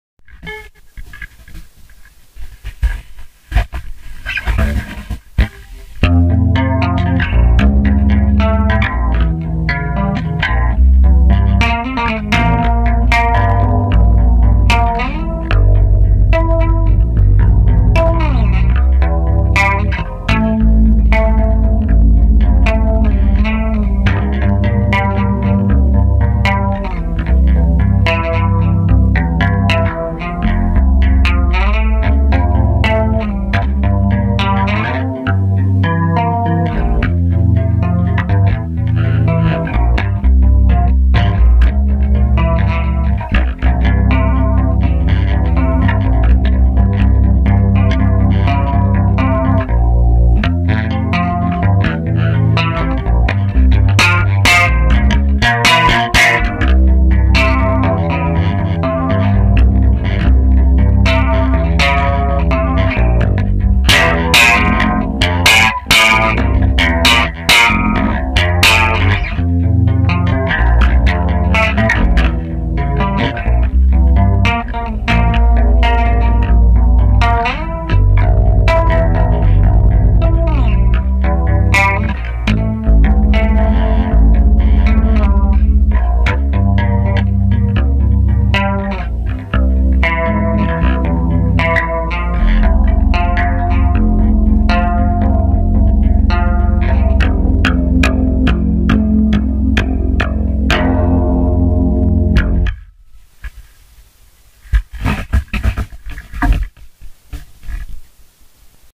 Bass Solo
Recorded on my new camera with a direct input so should sound better than my last upload attempt